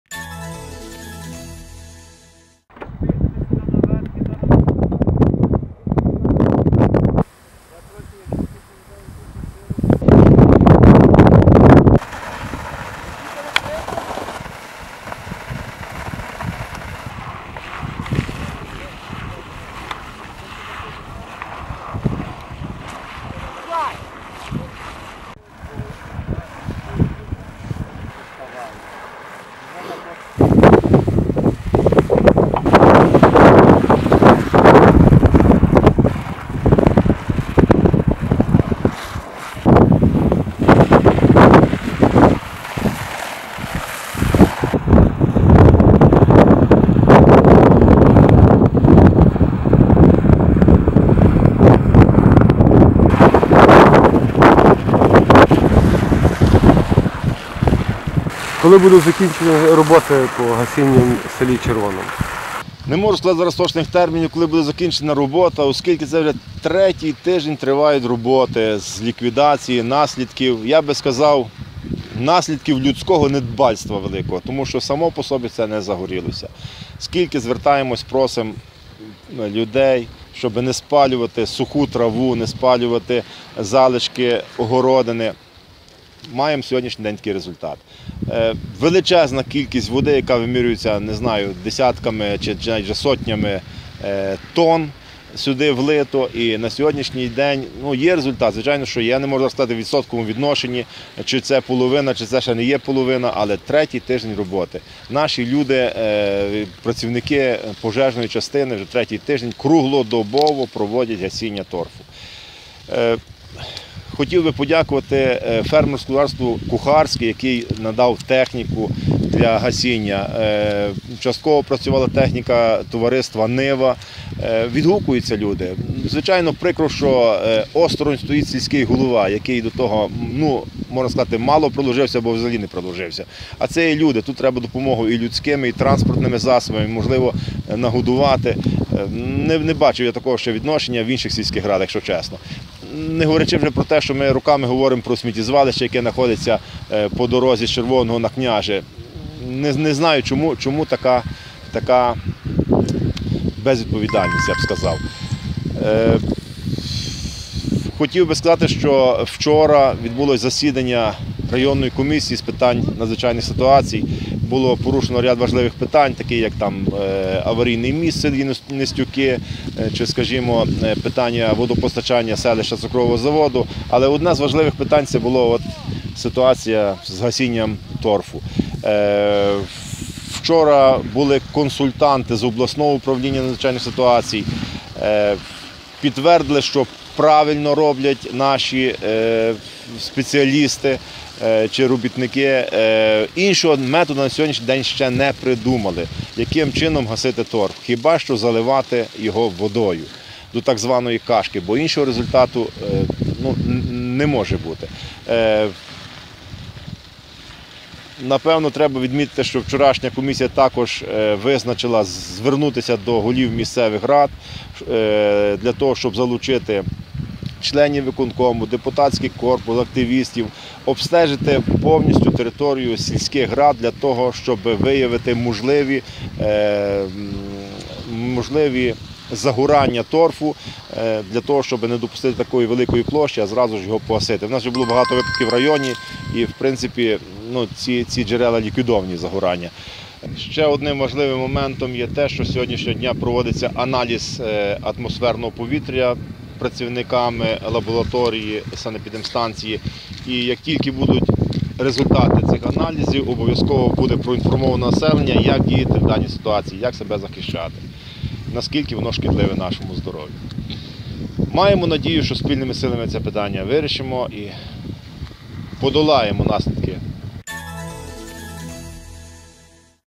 Золочівські рятувальники продовжують боротьбу з горінням торфу
Золочівські рятувальники цілодобово продовжують гасити пожежу на торфовищах, що на території Червоненської сільської ради.

Для гасіння пожежі витрачено величезну кількість води.

Вчора були консультанти із обласного управління із надзвичайних ситуацій, які підтвердили, що все правильно роблять золочівські рятувальники для  боротьби з горінням торфу.

Санепідемстанція проводить аналіз повітря.

Ситуацію із горінням торфу "Золочів.нет" прокоментував голова Золочівської РДА Володимир Недзельський.